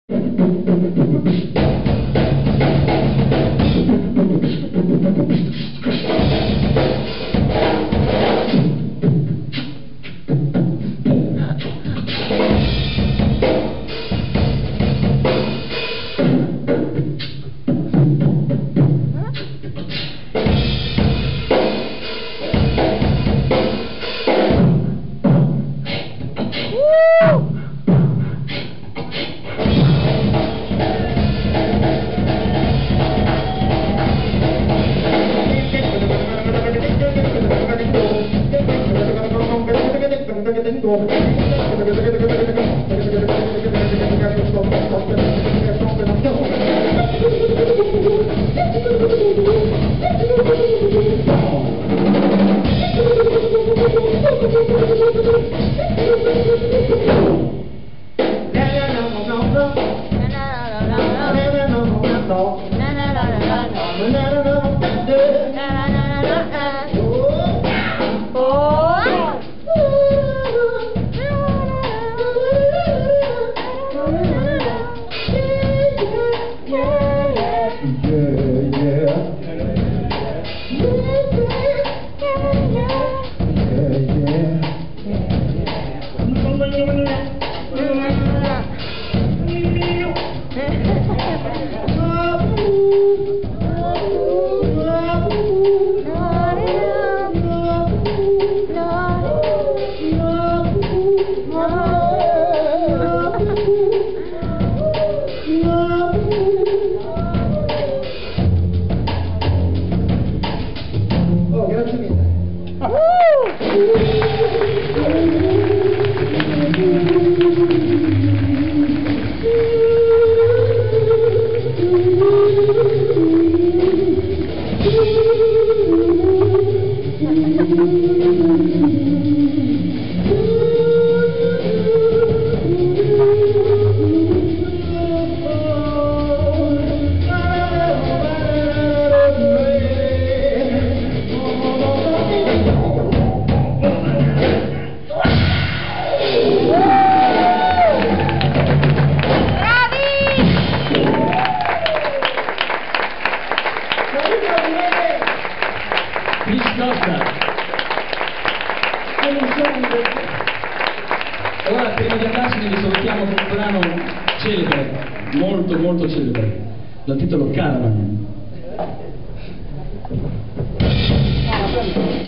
Fabio Nobile drum solo with Chris Costa @ Giulio Capiozzo Memorial 2009
Chris Costa vs Nobile